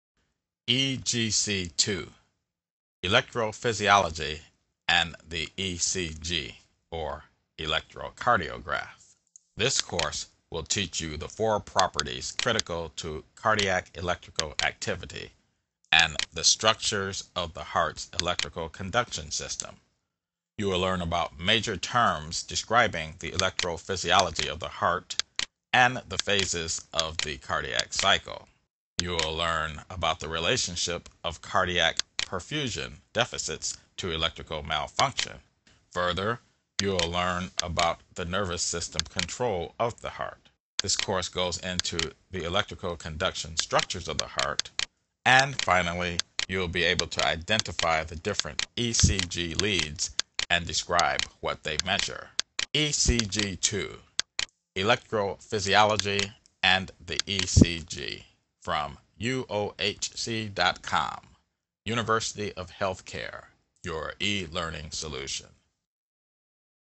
ECG 2 Electrophysiology & the ECG
Demonstrating the ECG 2 Electrophysiology & the ECG e-Learning course from uohc.com

(UniversityOfHealthCare & UniversityOfBusiness)